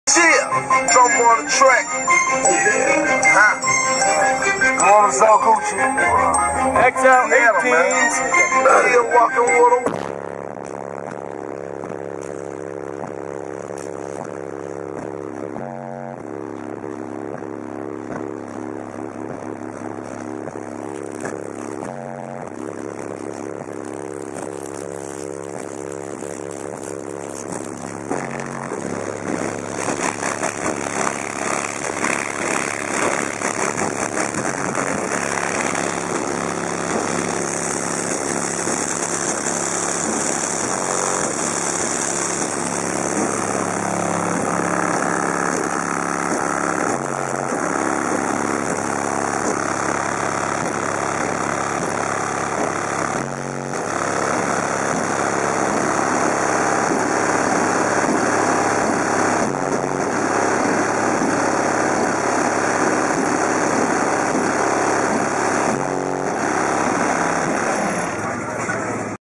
Josh Job Demoing His XL 18s
Josh was showing off his newest set-up while trying to show us all a bunch of flex. He succeeded :)